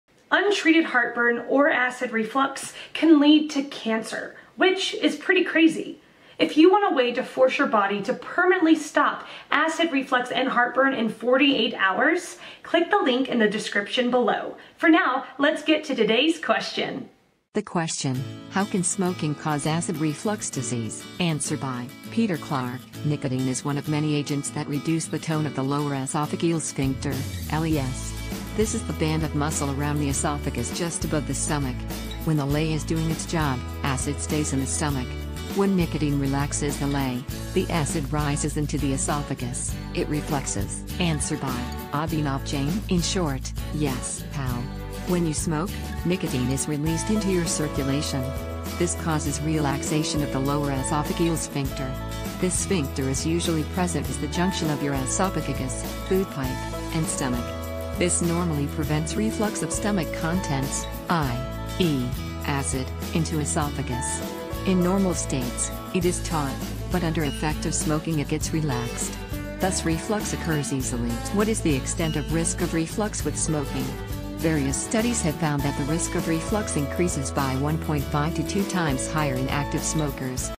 How Can Smoking Cause Acid Reflux Disease?
There is a way to force your body to kind of reset itself in about 48 hours

Nicotine is one of many agents that reduce the tone of the lower esophageal...